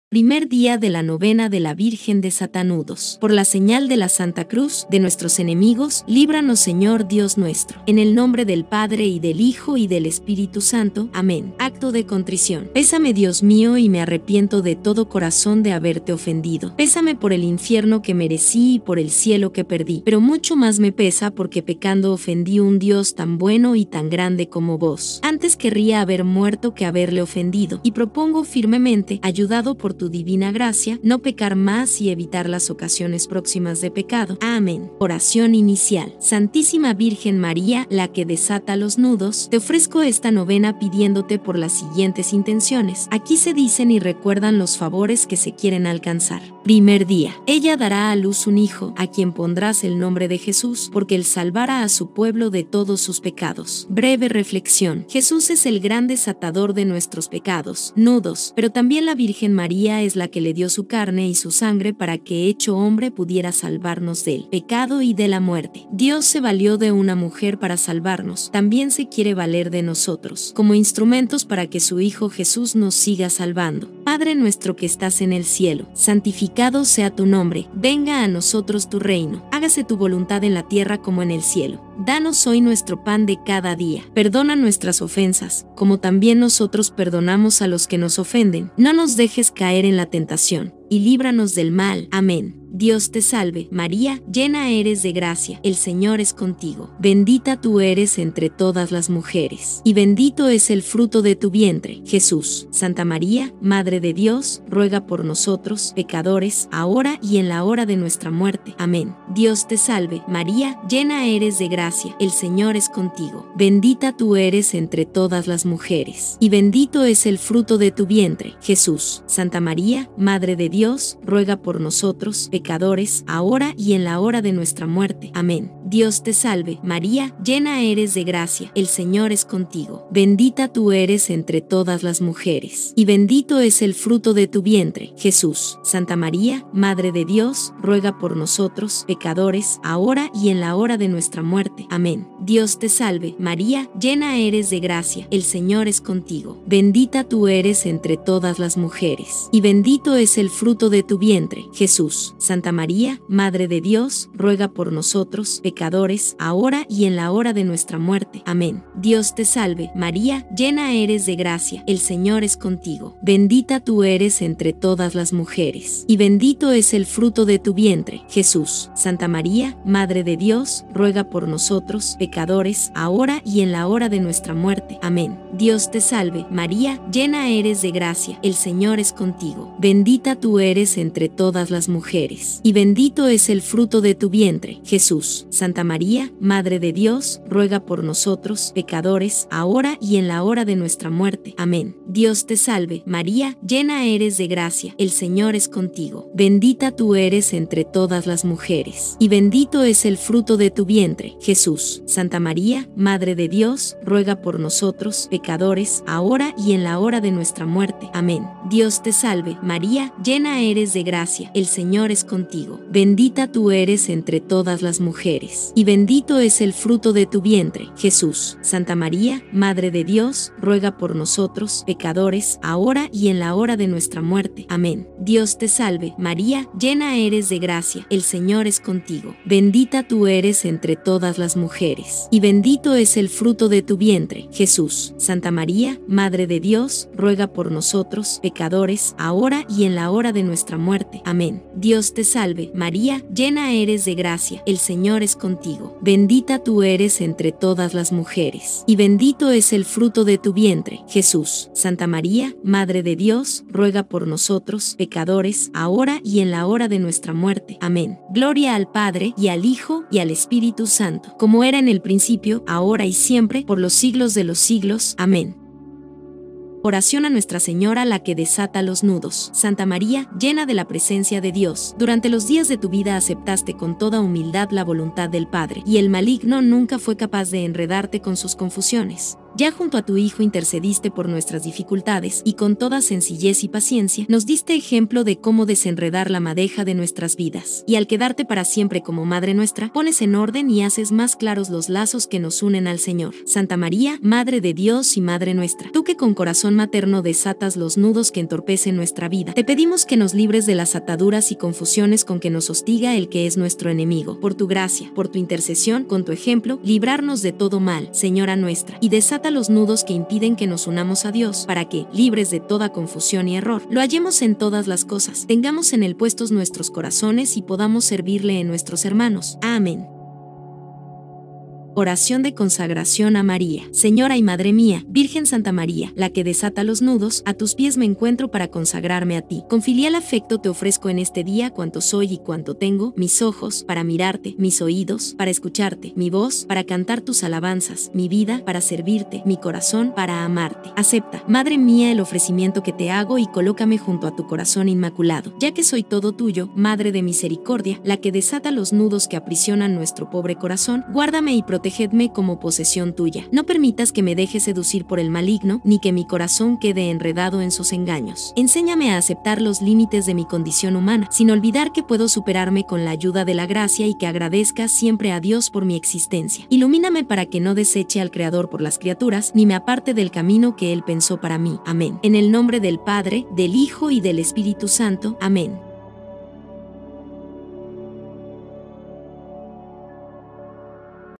Novena de la Virgen Desatanudos - Día 1 (Primer día)
La devoción a María Desatanudos ha tenido un alcance mundial desde finales del siglo XX, luego que el ahora Papa Francisco difundiera la imagen del cuadro de "María, desatanudos". Tras este hecho se empezó a realizar rápidamente una novena especial en honor a esta Virgen. La novena a María que desatanudos propone, durante nueve días, poner un nudo específico en las manos de María para que ella pueda interceder por nosotros ante Dios y alcanzarnos sus gracias.

Es bueno empezar por elegir la intención, el nudo que no podemos desatar y que deseamos confiar a María durante los nueve días de la novena. Este nudo puede ser cualquier dificultad que nosotros, o un ser querido tenga, esto puede incluir la dificultad espiritual, material, las pruebas de la vida, la enfermedad, la depresión, la adicción, una ruptura, problemas de la pareja, la búsqueda de un trabajo o una vivienda, etc. Algunos nudos son heridas profundas, otros son preocupaciones relacionadas con las situaciones que estamos atravesando.